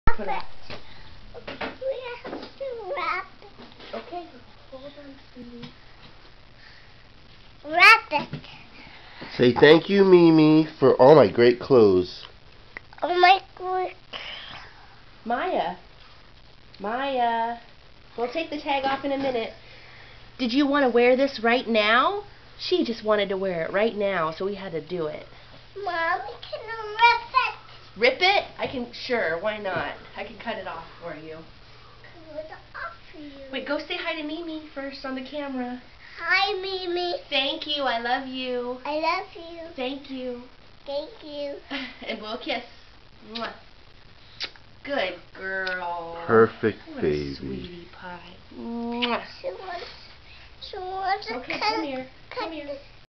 Maya loves her birthday gifts from Mimi!
She saw the skirt and said "I want to wear the orange!", so we put it on her right away.  Thank you Mimi!!